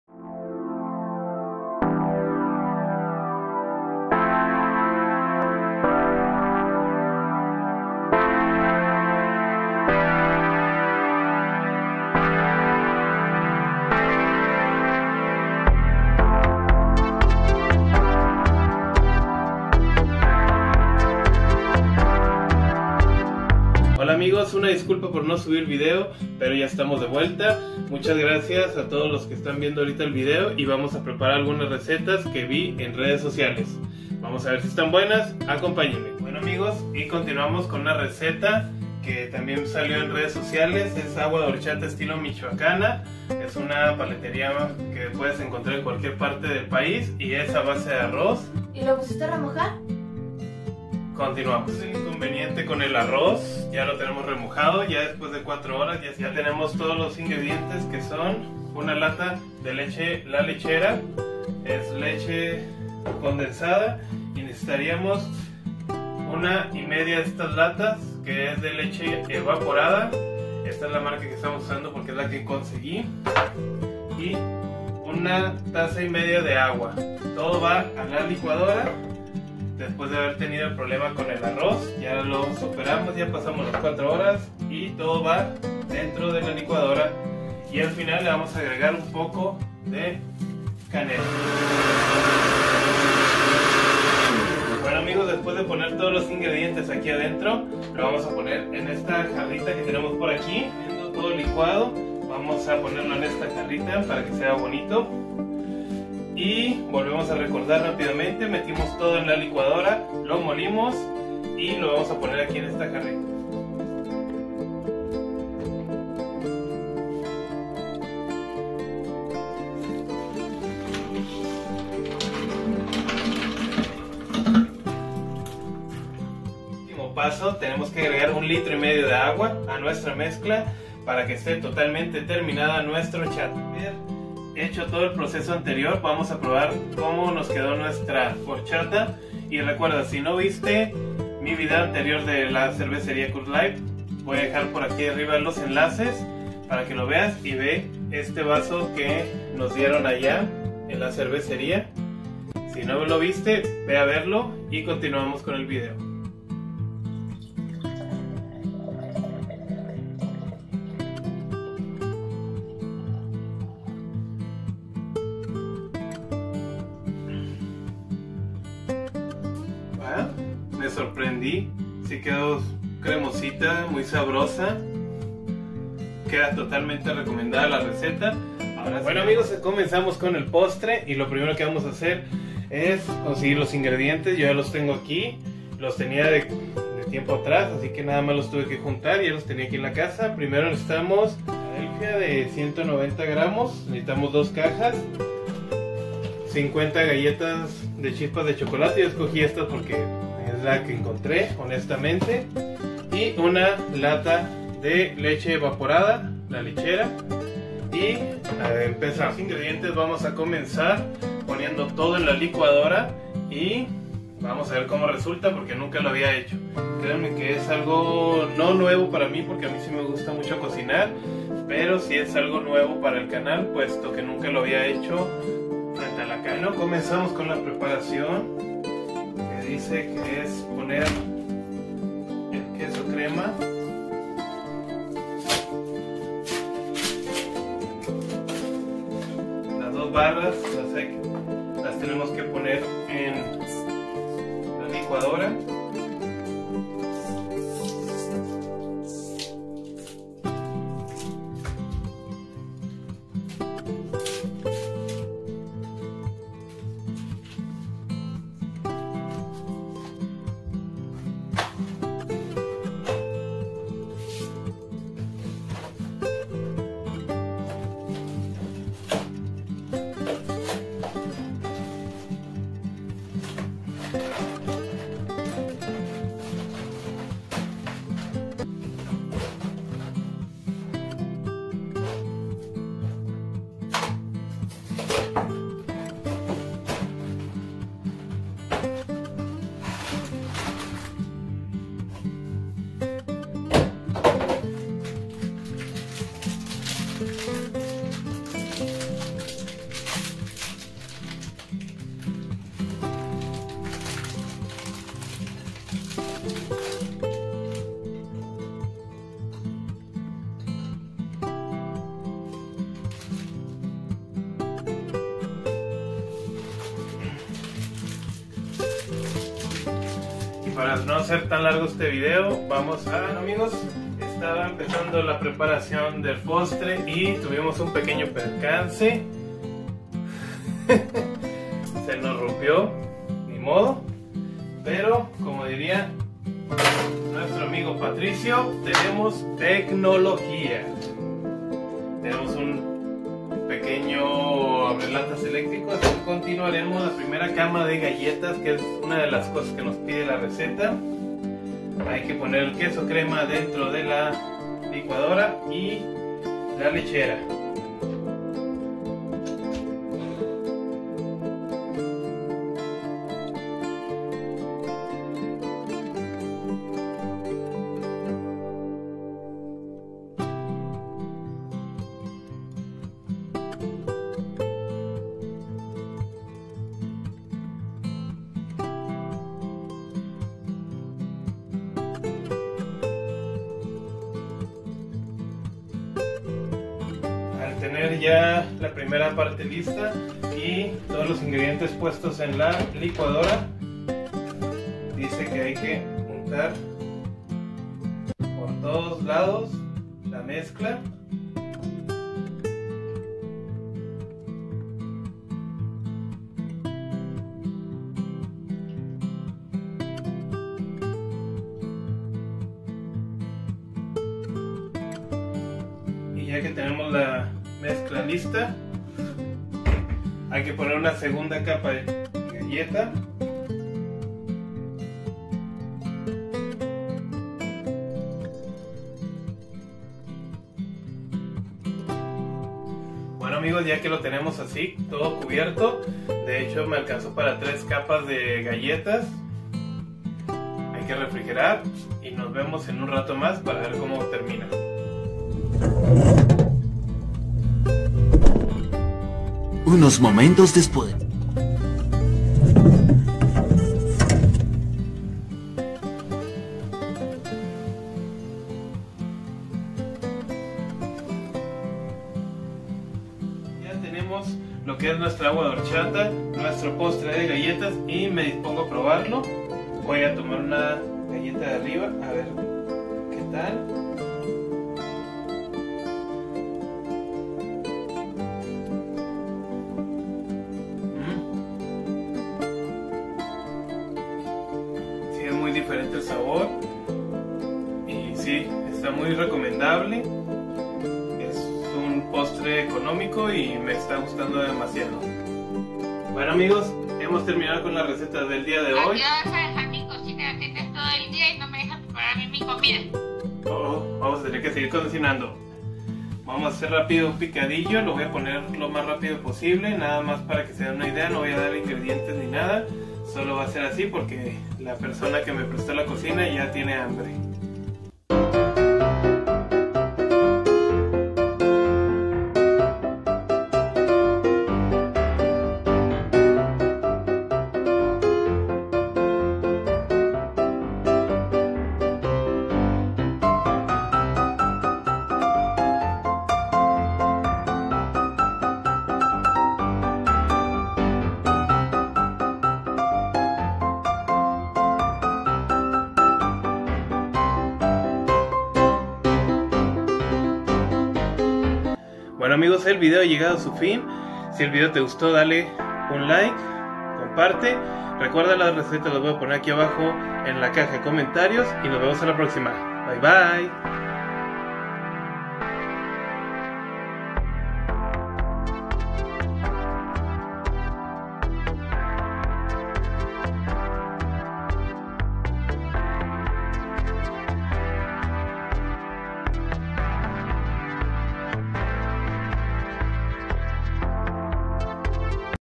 Postre de galletas y una rica horchata casera
Preparamos un postre de galletas y una rica horchata casera.
para toda la familia rico y facil

INGREDIENTES DE HORCHATA.
Para 4 personas
1 1/2 tazas Agua
1 taza Arroz
1 raja Canela
11/2 tazas Leche evaporada
1 taza Leche condensada
1 1/2 It Agua
2 tazas Hielos
Canela en polvo
PROCEDIMIENTO PARA EL ARROZ
REMOJA el arroz y la canela en el agua durante 4
horas.
LICUA el arroz con el agua de remojo, la canela, la
leche evaporada, la leche condensada y una taza de
agua hasta triturar la canela
.VIERTE en una jarra y añade el resto del agua.
MEZCLA hasta integrar.
SIRVE el hielo en vasos y vierte la horchata.
ESPOLVOREA canela en polvo.

Cheesecake de galletas chokis
Necesitas:
1 lechera
2 barritas de queso crema de 190 gramos
50 galletas
chokis
Procedimiento:
1) agrega el queso crema y la lechera a la licuadora
2) En un refractario pones una capa de galletas y arriba una
capa del queso y la lechera luego haces lo mismo 2 veces.
3) Al final lo adornas con galletas quebradas y puedes agregar
chispas de chocolate si gustas.
Refrigera 4 horas
y disfruta
Fácil, Rápido